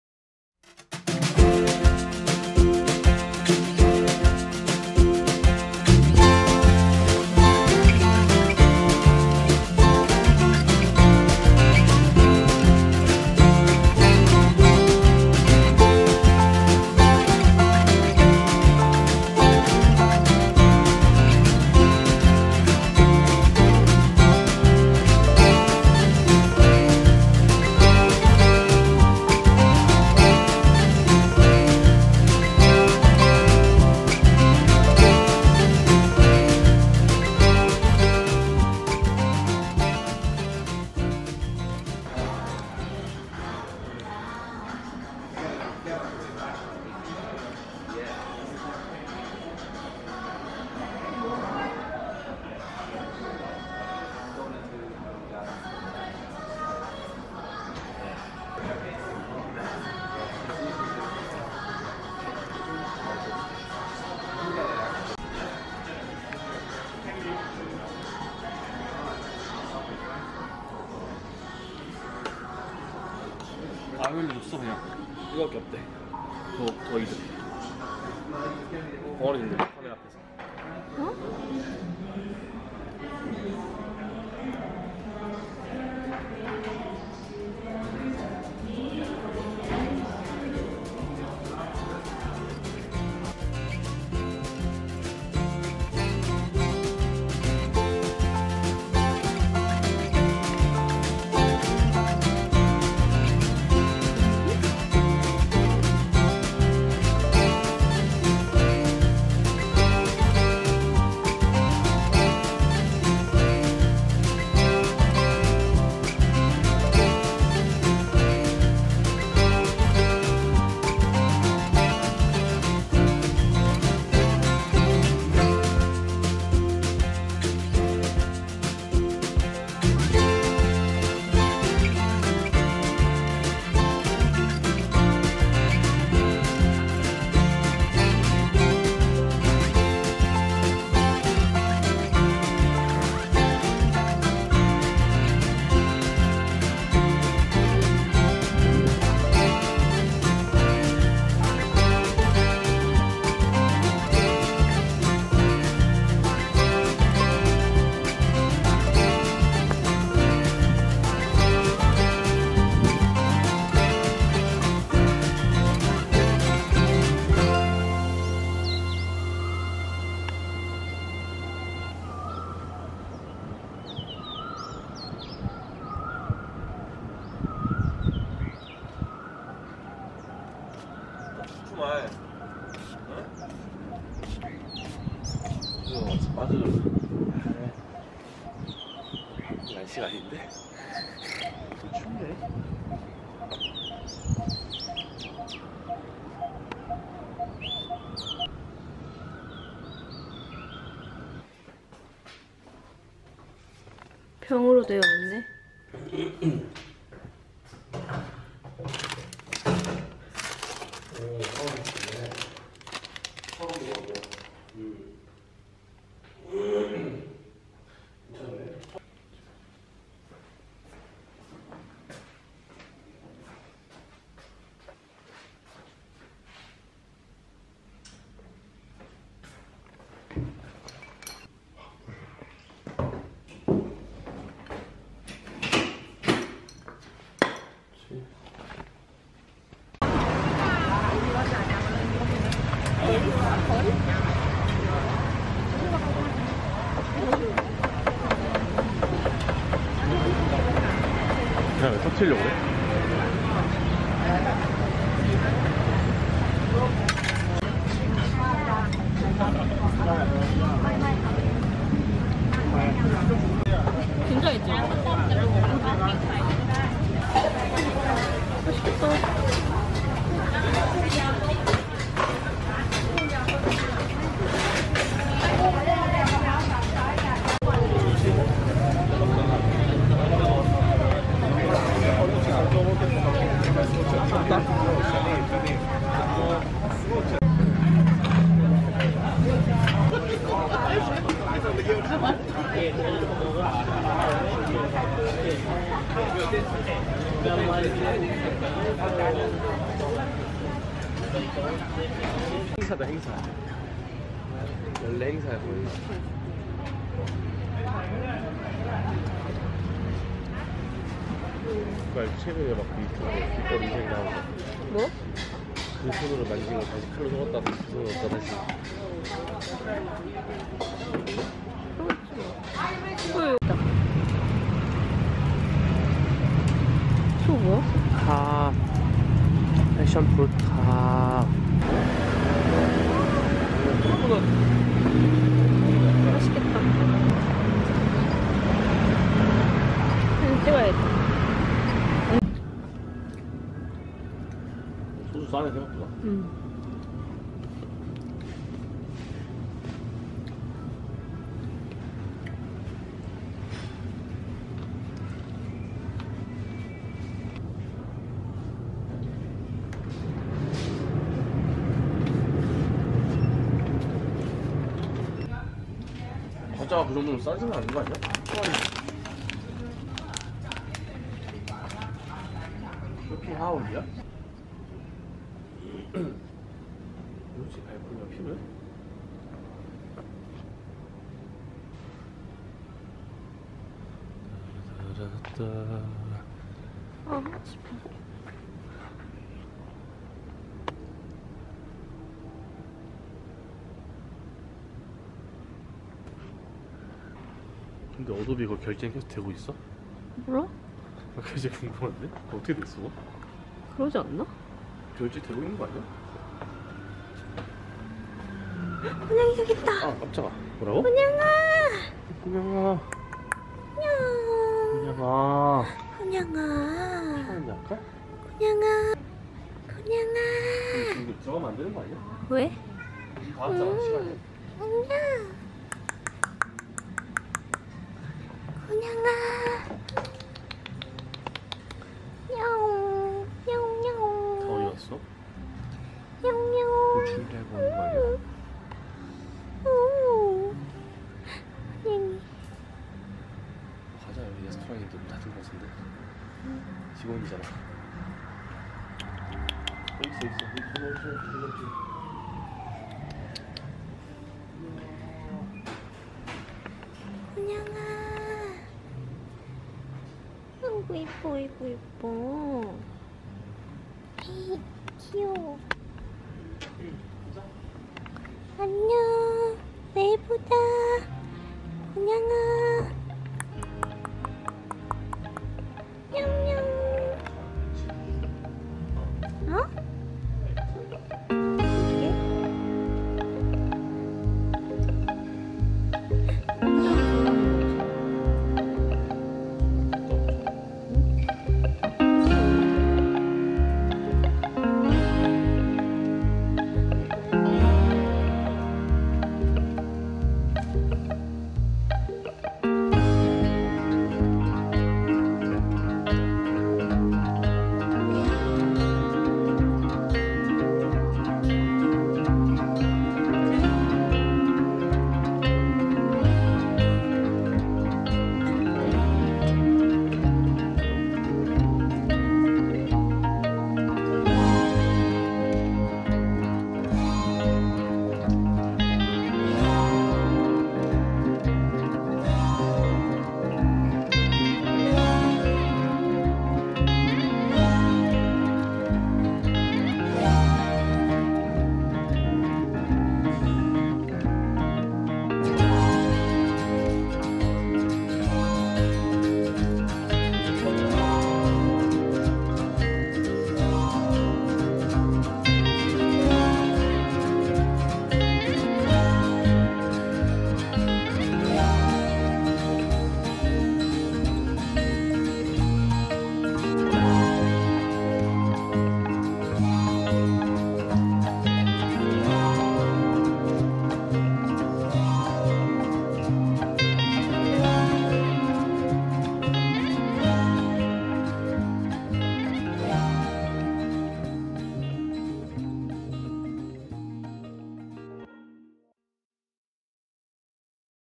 태국 치앙마이_첫번째 이야기[그린타이거 하우스/현지 마켓 탐방]
2년전에 여행했던 치앙마이 영상이에요! 이게 마지막 해외여행이 될거라고는 상상도 못했어요...ㅠ 다시 여행을 꿈꾸며 영상을 올려봅니다.